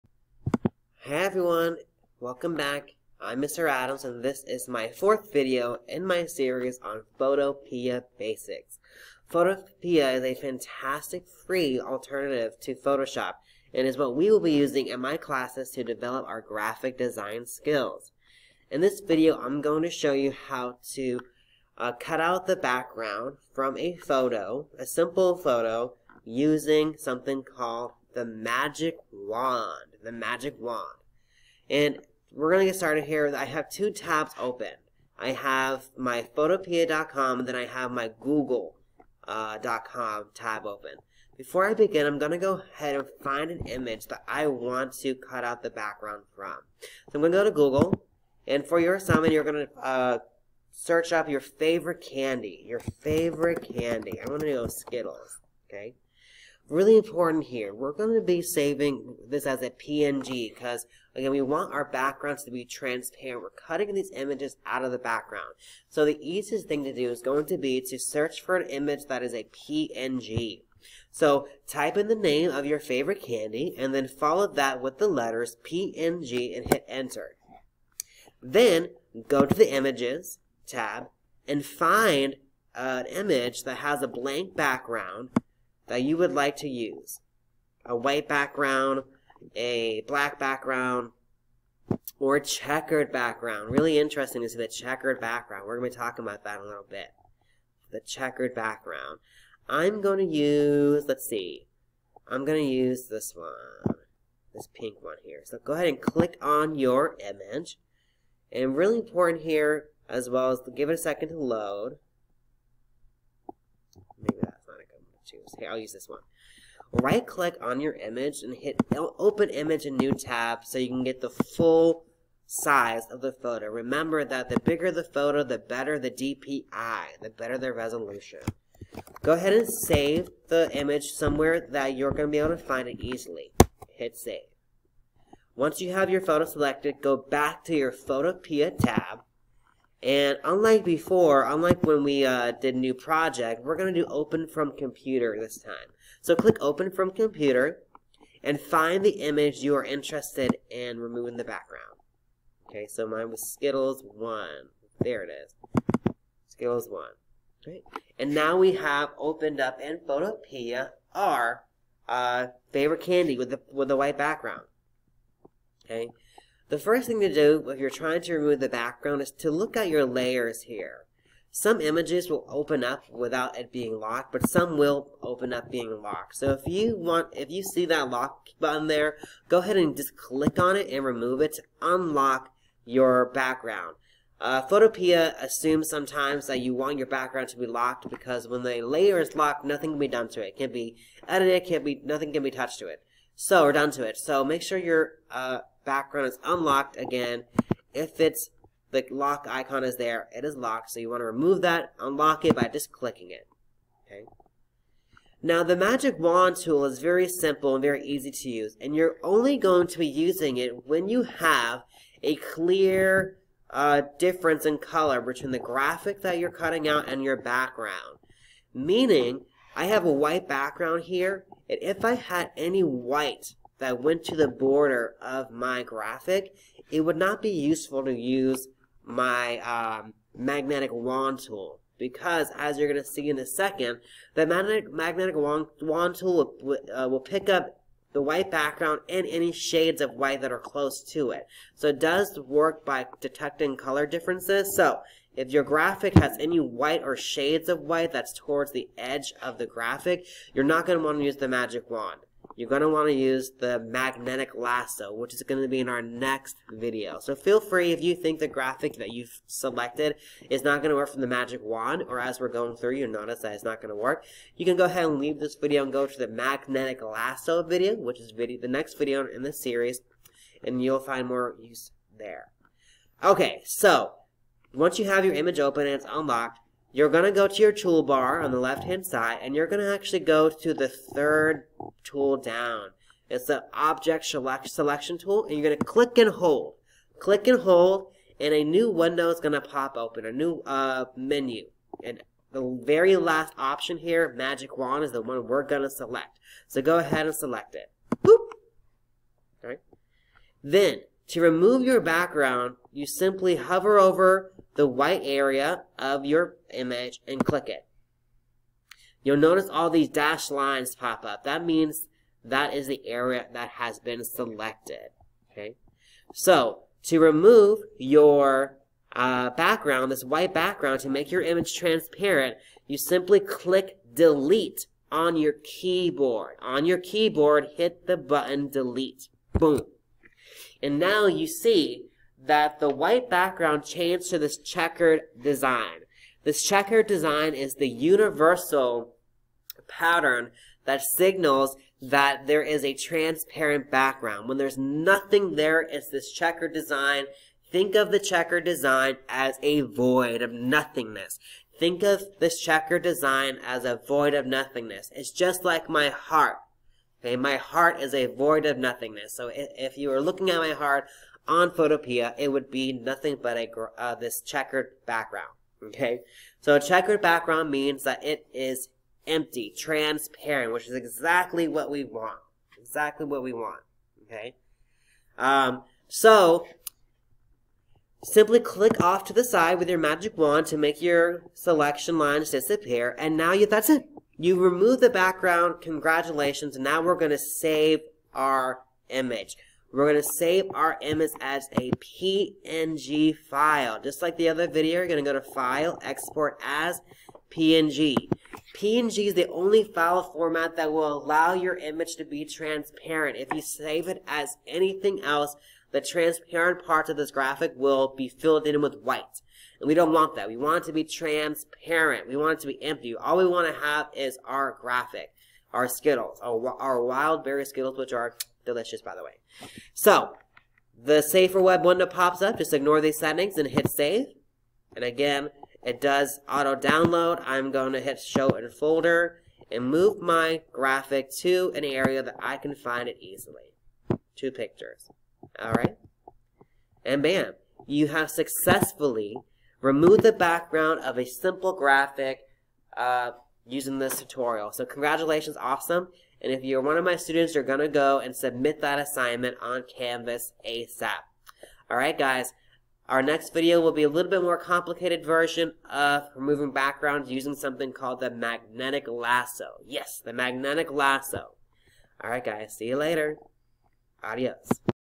Removing Background with Magic Wand
Video #4: Removing the background of a simple image or PNG utilizing the magic wand tool in Photopea.com